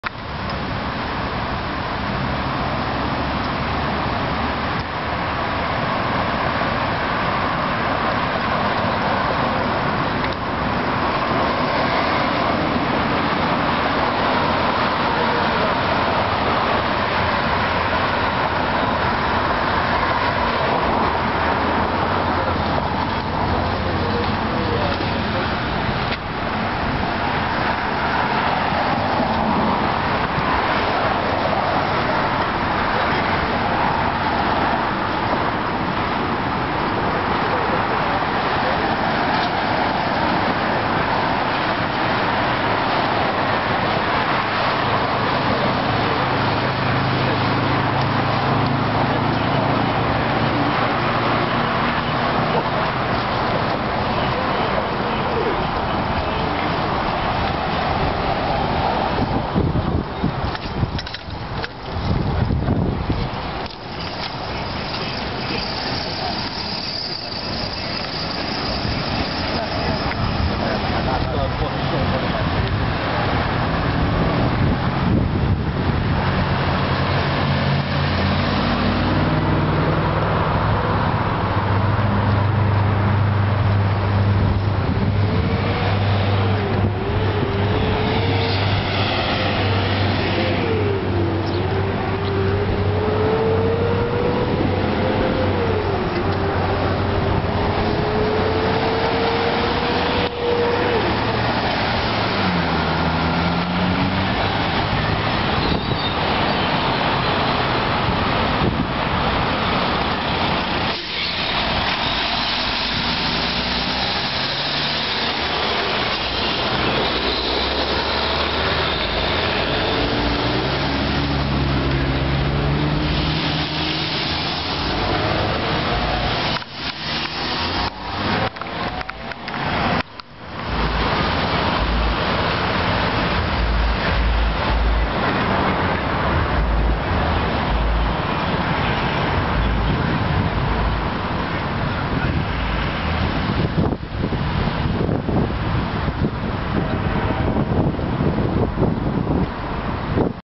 Crossing Lakeshore to Strachan
For a cyclist coming off the Martin Goodman Trail in Toronto, this is what they face when trying to get to the bike lanes on the other side of the intersection at Lakeshore and Strachan.

There is no way for someone to go northbound legally on their bike. There is no light to let you know when you are allowed to cross. Instead people take the crosswalk on the west side. The crosswalk, while not legal (on bike), is at least safer than trying to figure out when you have the right to cross.